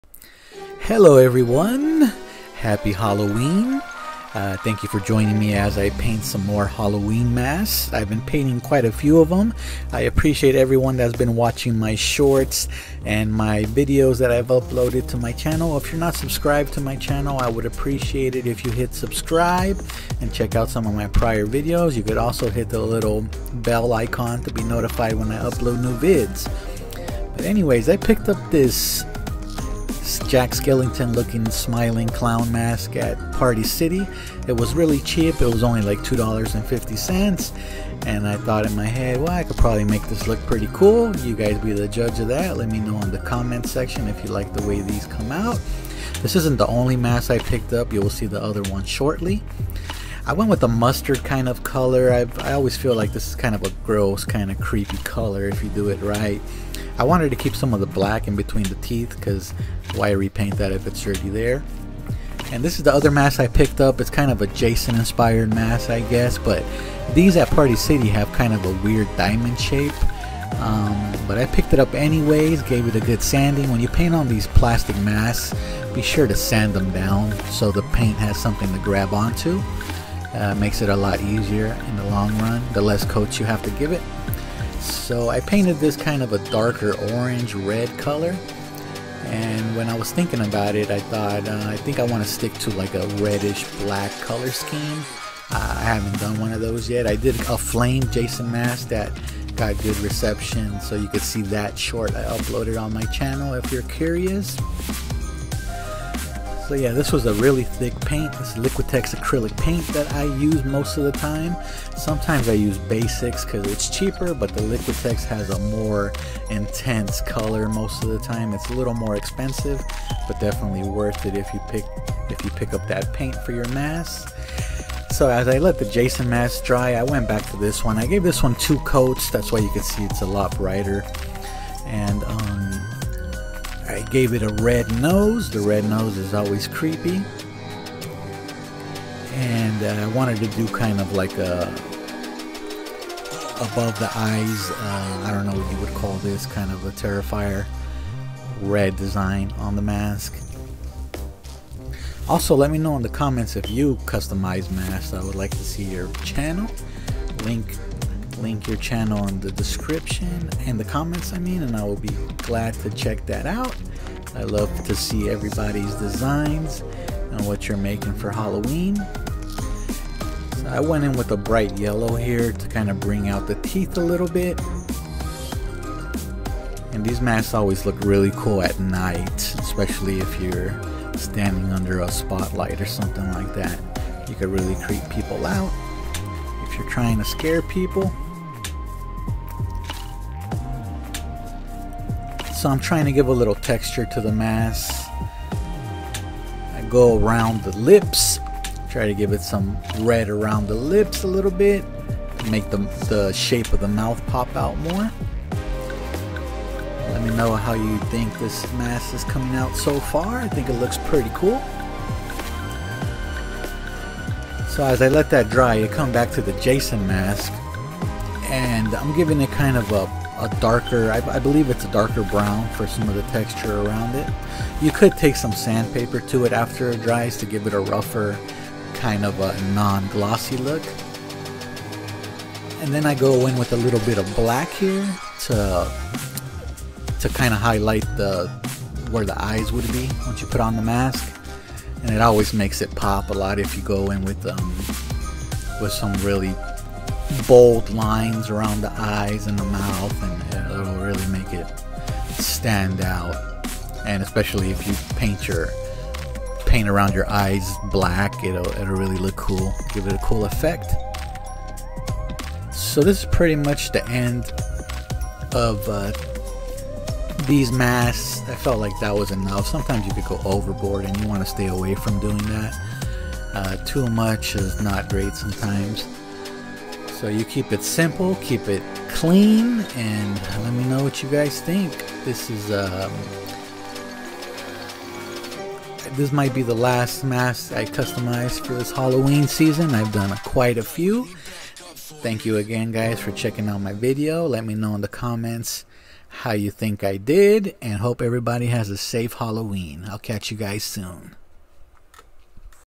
Upgrading a cheap Halloween Mask from Party City!?
In this video i custom paint a cheap Halloween Mask from Party City. As a bonus i did a second mask for you!